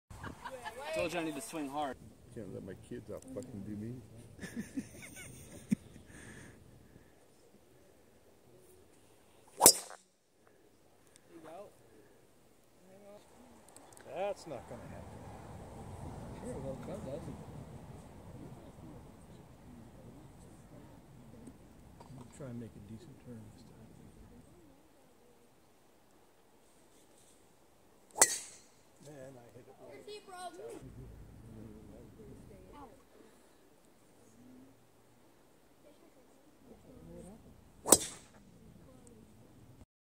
Gramps hitting the ball super far
He is a pro golfer and every time I go to his house I get to drive the golf cart